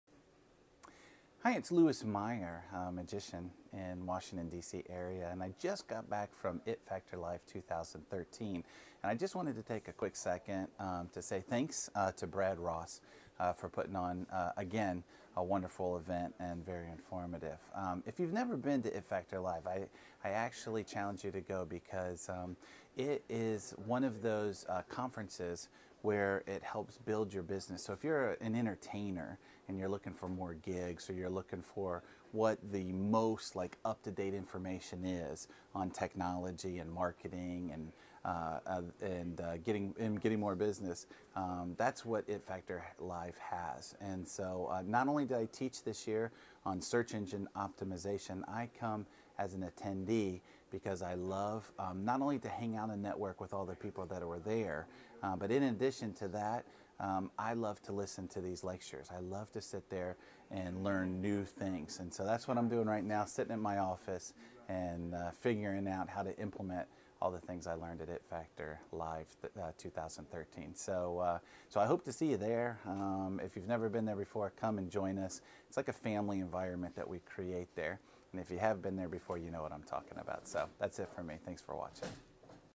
Louis the Magician speaks about It Factor Live 2013
Speaking about my experience at It Factor Live 2013